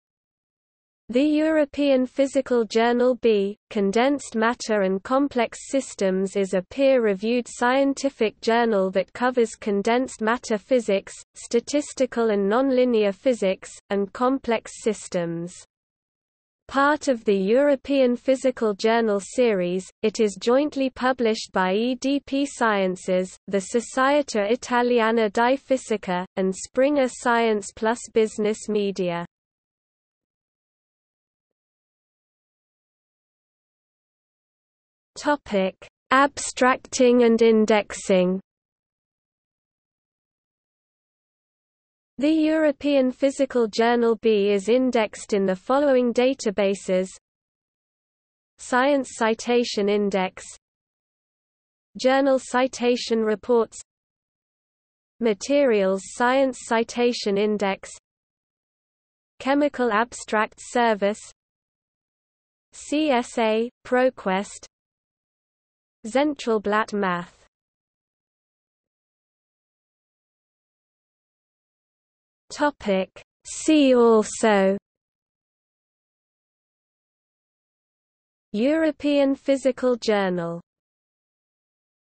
The European Physical Journal B | Wikipedia audio article
00:00:31 Abstracting and indexing
00:01:08 See also

- Socrates

SUMMARY
The European Physical Journal B: Condensed Matter and Complex Systems is a peer-reviewed scientific journal that covers condensed matter physics, statistical and nonlinear physics, and complex systems.  Part of the European Physical Journal series, it is jointly published by EDP Sciences, the Società Italiana di Fisica, and Springer Science+Business Media.